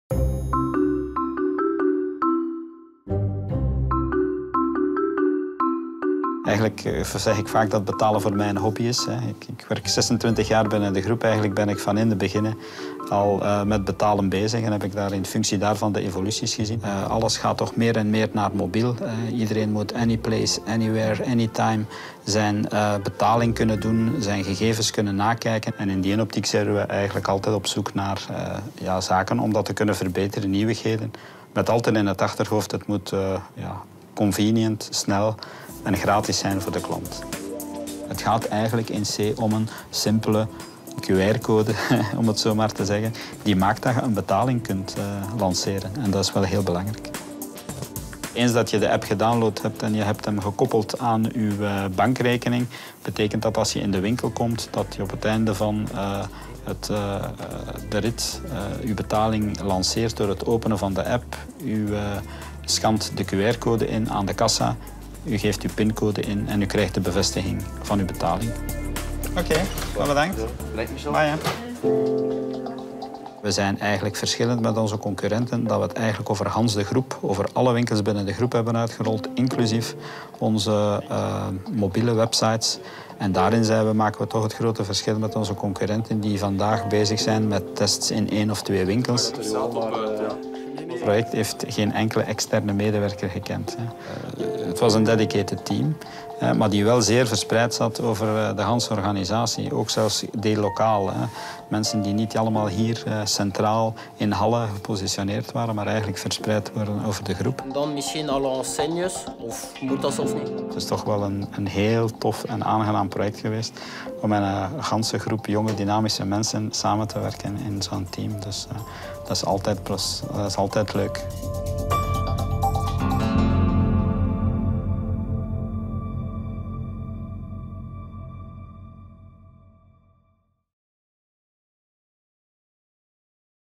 Colruyt Group introduces mobile payment to Belgium
More and more people own a smartphone. That is why Colruyt Group introduced a new payment method: the payment application SEQR. You can use this to pay at Colruyt, OKay, Spar, Dreamland, Dreambaby, Bio-Planet, and ColliShop. This allows you to pay quickly, easily, and safely with your smartphone at all the Belgian store formulas. And all that thanks to Michael's team.